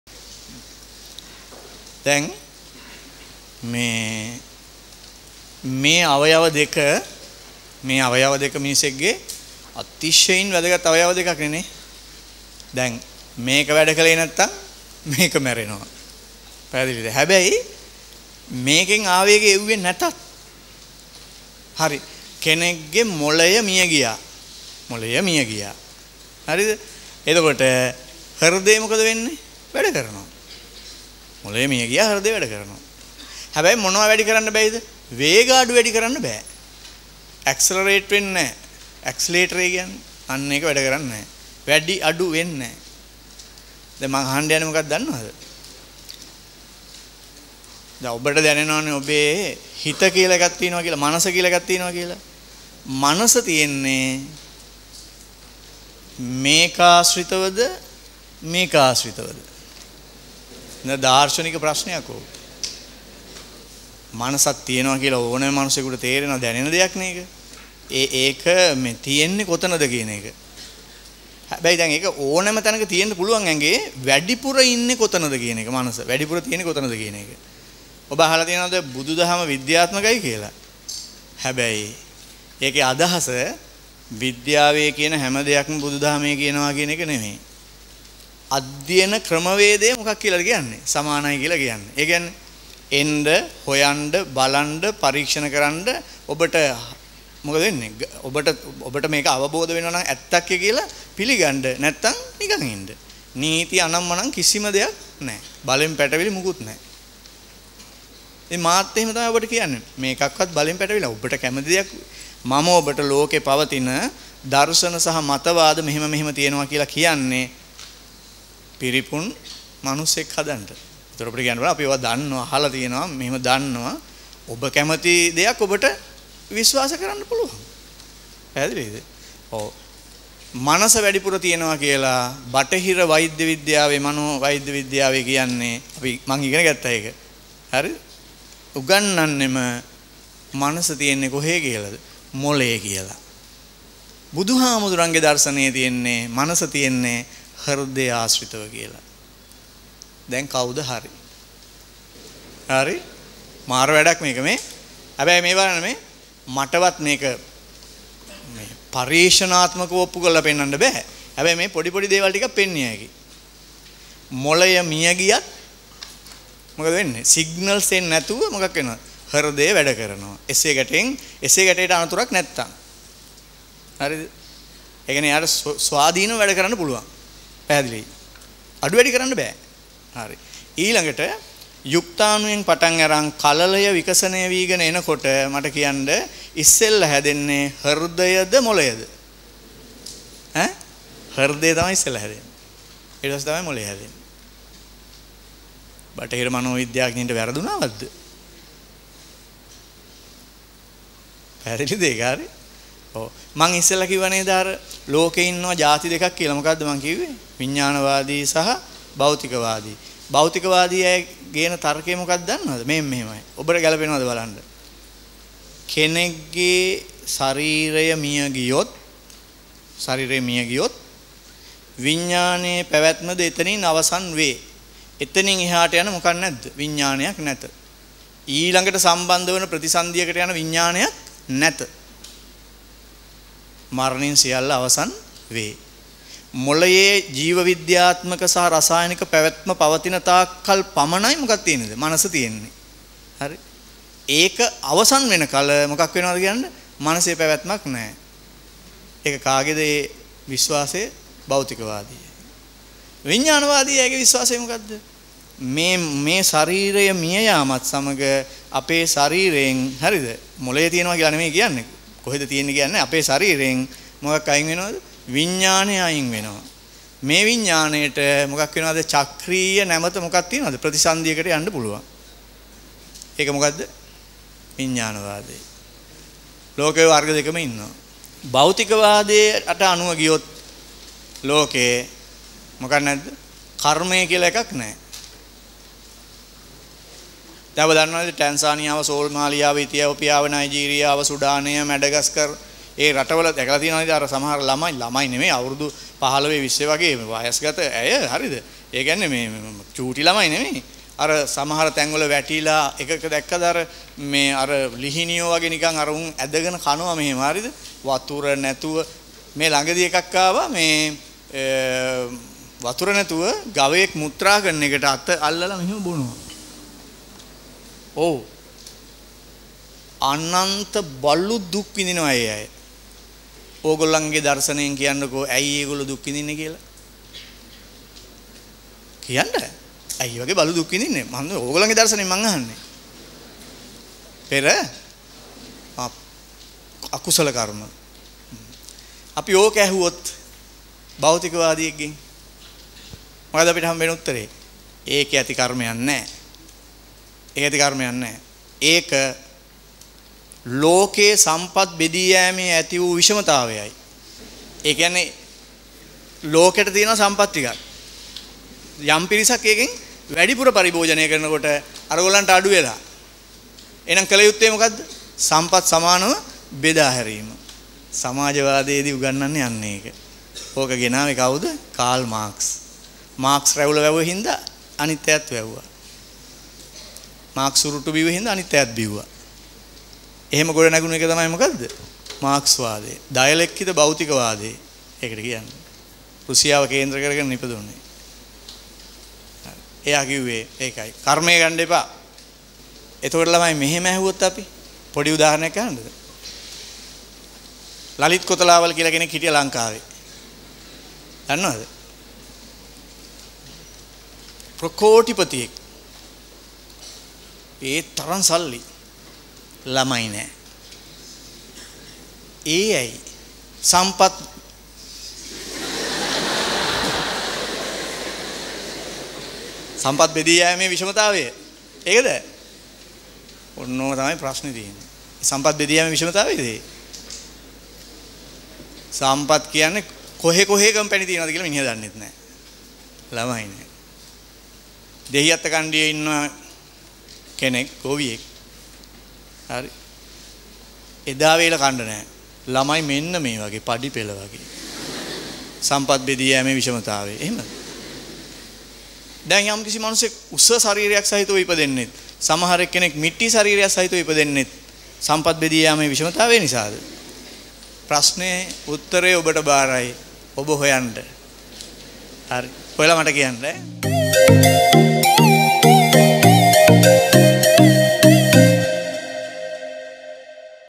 Tissa Jananayake - Episode 17 | Anomaly in resource allocation | සම්පත් බෙදී යාමේ විෂමතාවය
- මොලය සහ හදවත කියන්නේ අතිශය වැදගත් අවයව දෙකක්
- මනස තියෙන්නේ මොලය ආශ්‍රිතව ද? හදවත අශ්‍රිතව ද?
- බුදුදහම විද්‍යාත්මකයි කියන්නේ බුදුදහමේ තියන හැමදෙයක්ම විද්‍යාවේ කියනවා කියන එක නෙමේ
- මම ඔබට ලෝකේ තියන දර්ෂණ හා මතවාද ගැන කියන්නේ පිරිපුන් මනුස්සයෙක් හදන්න
- ඔබට කැමති නම් පිළිගන්න පුලුවන්න, නැත්නම් ප්‍රතික්ෂේප කරන්න පුළුවන්, බලෙන් පැටවීමක් නෑ
- ලෝකේ ඉන්නවා ජාති දෙකක්, භෞතිකවාදීන් සහ විඥානවාදීන්
- මාක්ස් රැවුල වවපු නිසා එයාලත් රැවුල වැවුවා
- කෙන්ක් උස ශරීරයක් සහිතව ඉපදෙන්නෙත්, මිටි ශරීරයක් සහිතව ඉපදෙන්නෙත්, සම්පත් බෙදී යාමේ විෂමතාවය නිසාද?